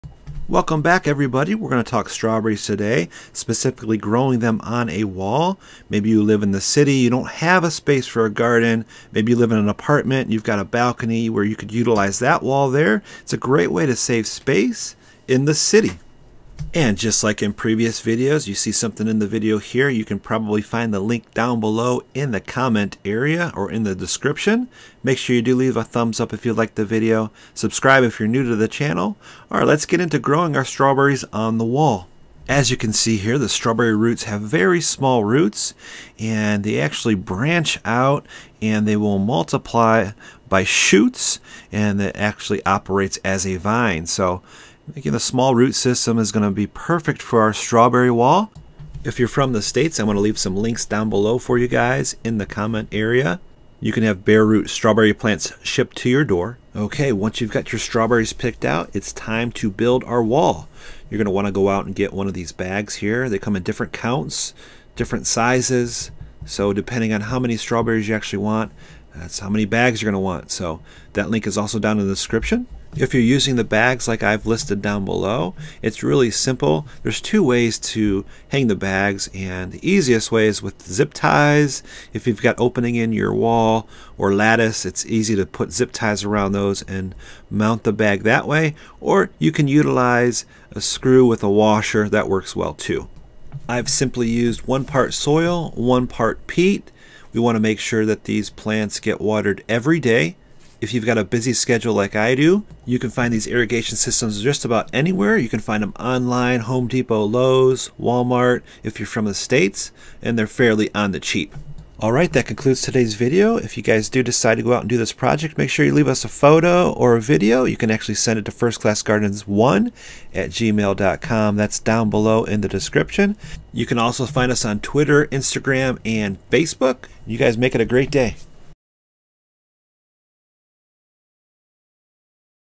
How To Grow Strawberries on Your Wall (Growing Strawberry Vine)
Growing a Ton of Strawberries on a wall or fence is easy from home even in the city in containers!  Plant Strawberry wall can save space or grow them in a small space.

Try this grow a ton of strawberries on a wall at home.  Use the garden bags to easily create a wall of strawberry.  Easy to do and great for small spaces to hang strawberries in containers on your fence. how to grow strawberries vine